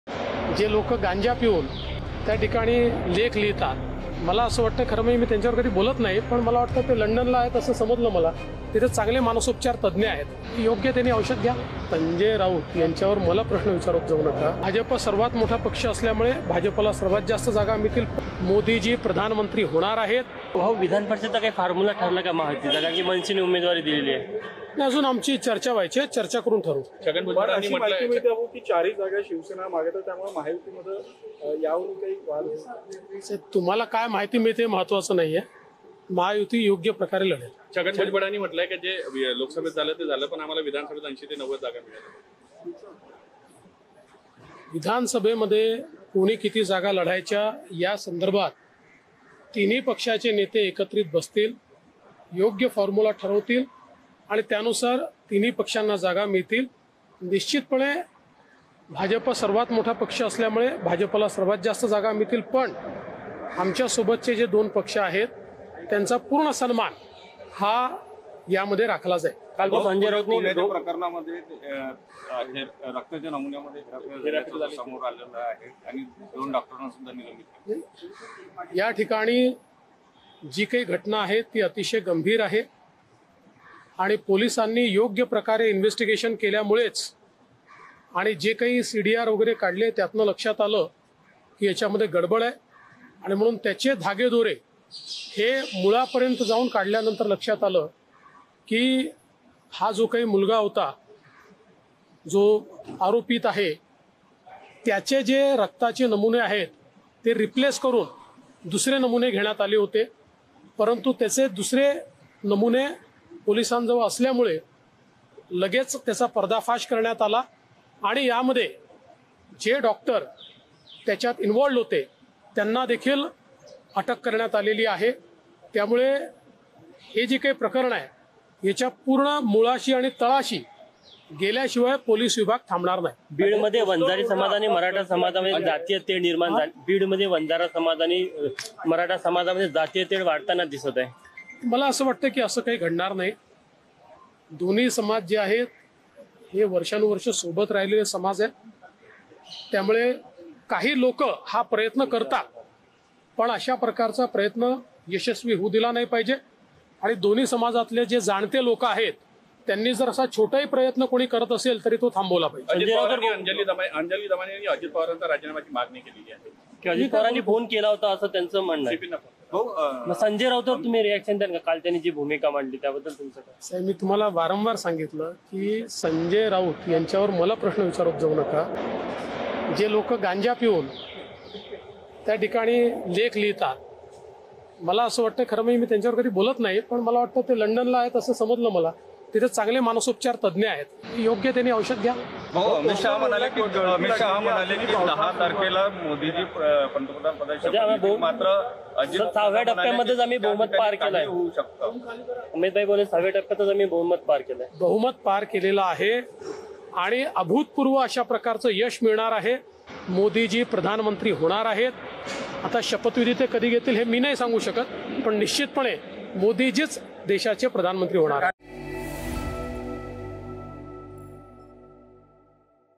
Devendra Fadnavis : संजय राऊत यांनी मानसोपचार तज्ज्ञांकडे जाऊन उपचार घ्यावा - देवेंद्र फडणवीस
Devendra Fadnavis On Sanjay Raut  : संजय राऊत यांनी मानसोपचार तज्ज्ञांकडे जाऊन उपचार घ्यावा - देवेंद्र फडणवीस

राज्याचे उपमुख्यमंत्री देवेंद्र फडणवीस यांनी ठाकरे गटाचे खासदार संजय राऊत यांच्यावर जोरदार टीका केला आहे. देवेंद्र फडणवीस म्हणाले की, संजय राऊत यांच्याबद्दल मला प्रश्न विचारू नका. राऊत हे गांजा पिऊन लेख लिहितात. राऊत हे सध्या लंडनला आहेत, त्यांनी तिथे मानसोपचार तज्ज्ञांकडे जाऊन उपचार घ्यावा, असं फडणवीस म्हणाले.